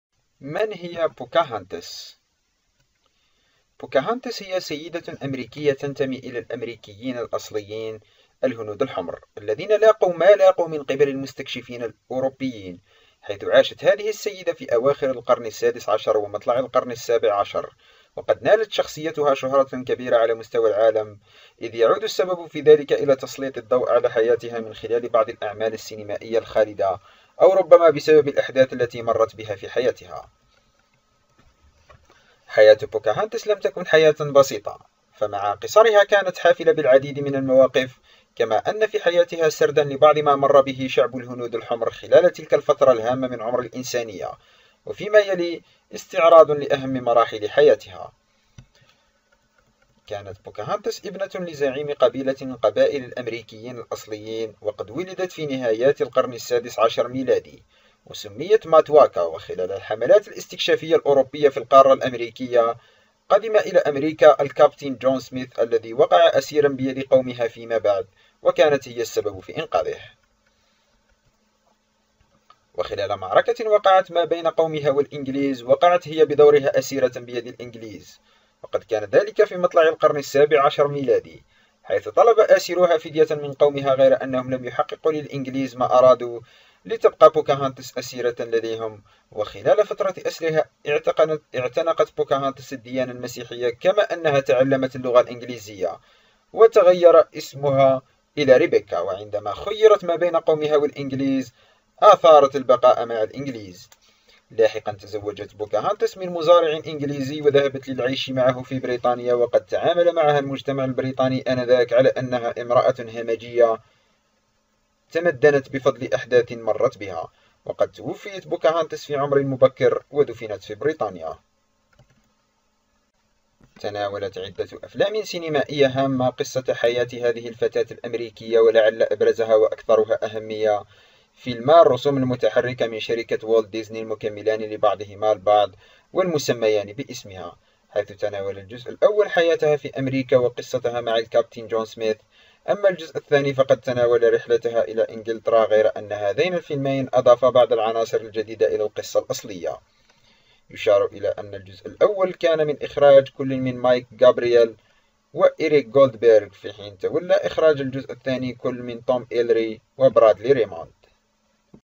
من هي بوكاهانتس ؟
بوكاهونتاس هي سيدة أمريكيّة تنتمي إلى الأمريكيين الأصليين (الهنود الحمر) الذين لاقوا ما لاقوا من قبل المستكشفين الأوروبيين، حيث عاشت هذه السيدة في أواخر القرن السادس عشر ومطلع القرن السابع عشر، وقد نالت شخصيّتها شهرة كبيرة على مستوى العالم، إذ يعود السبب في ذلك إلى تسليط الضوء على حياتها من خلال بعض الأعمال السينمائيّة الخالدة، أو ربما بسبب الأحداث التي مرت بها في حياتها. Volume 0%